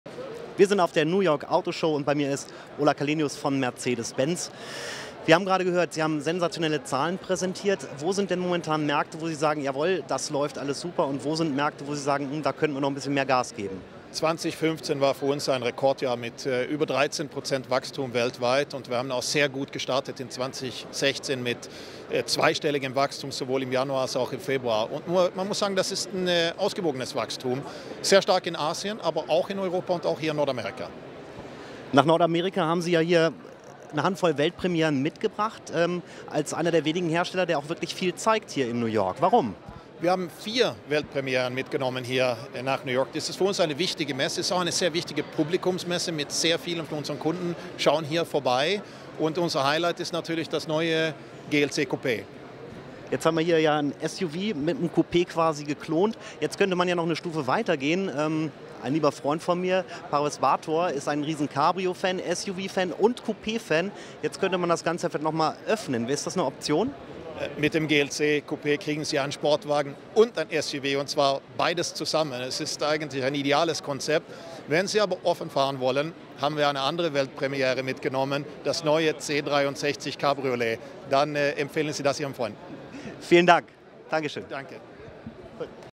Mercedes in New York (2016) - Interview mit Ola Källenius
Wir sprechen exklusiv mit Ola Källenius über Daimlers neu vorgestellten Modelle in New York. Mercedes kommt gleich mit vier Weltpremieren. Unter anderem dem GLC Coupé.